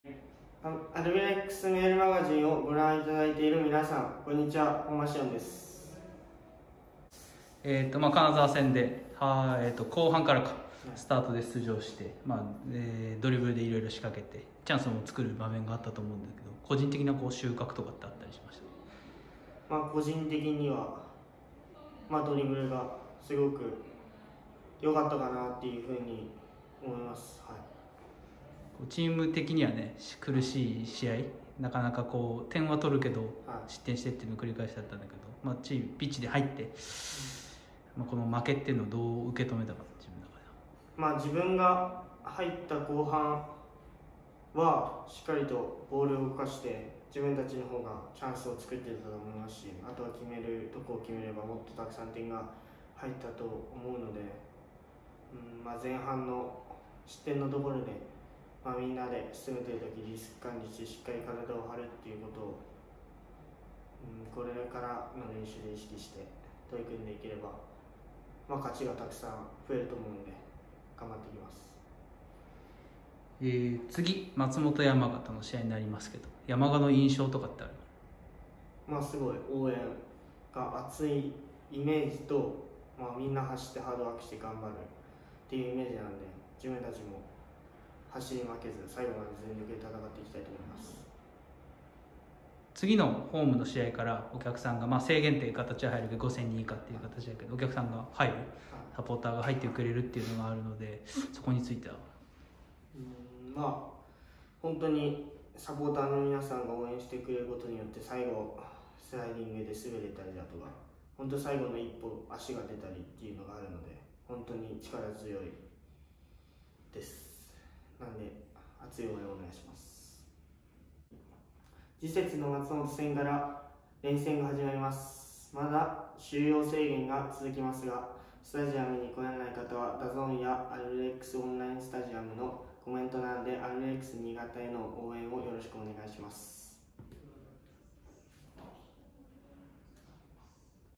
7月11日（土）はデンカビッグスワンで松本山雅FC戦! 本間至恩選手からメッセージ！
今週末7月11日（土）は2020明治安田生命Ｊ２リーグ・松本山雅ＦＣ戦！
今節から収容制限5,000人での試合となります。チケットはＪリーグチケット・ローソンチケットで販売中！
再開後初勝利に向けて、一丸となって戦います。
本間至恩選手からのメッセージをお届けいたします。